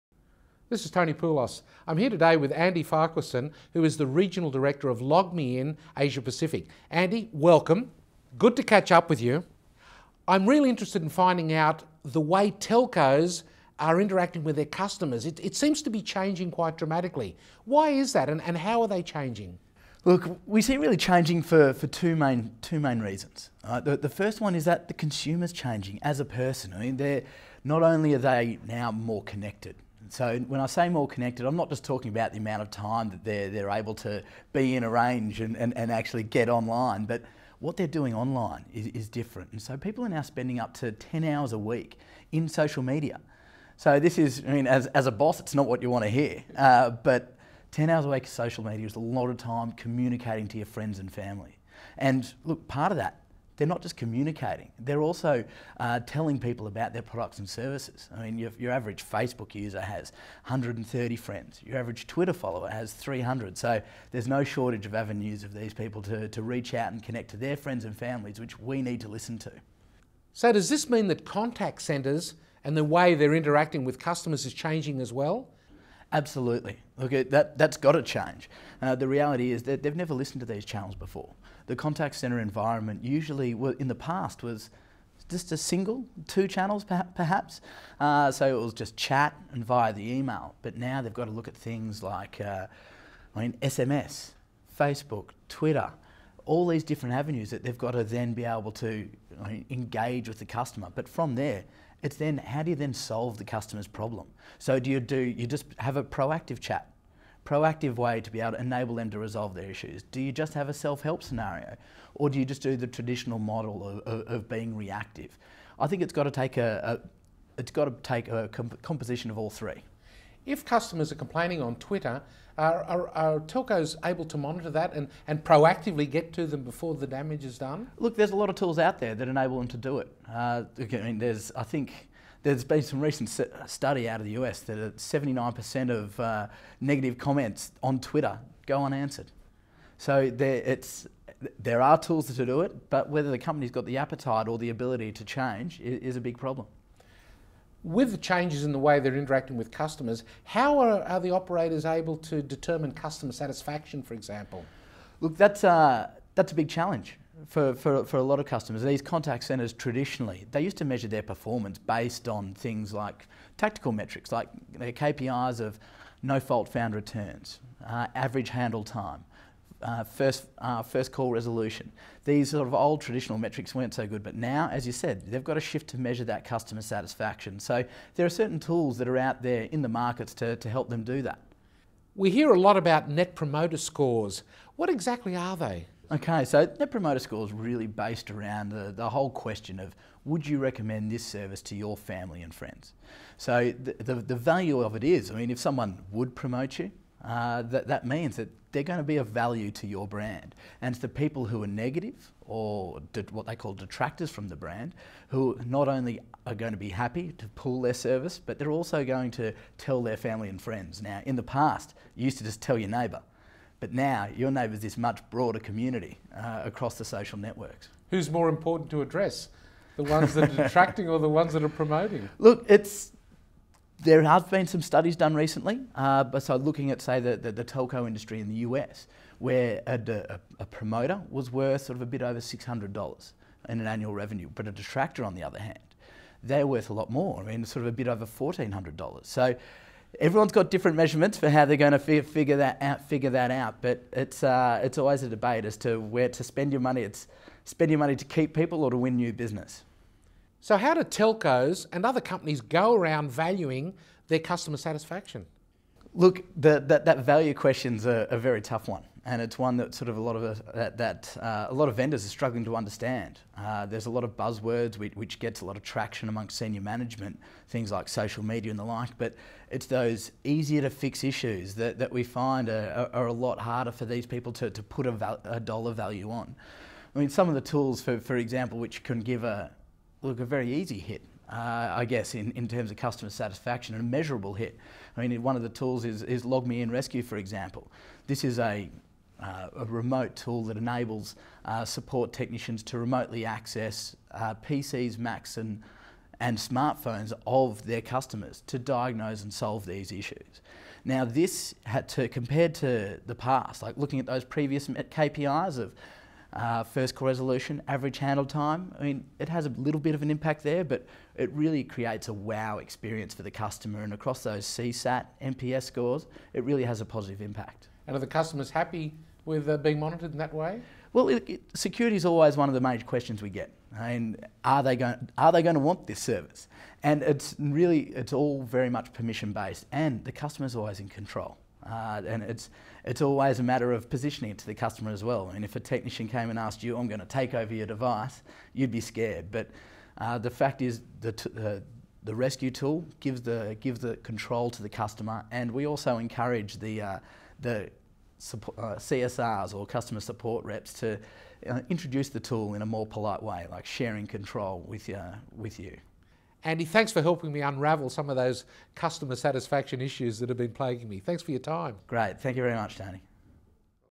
Customers are interacting differently these days, but how?
LogMeIn's Andy Farquharson explains how customer's expectations are changing and the challenge operators face in changing how they interact with them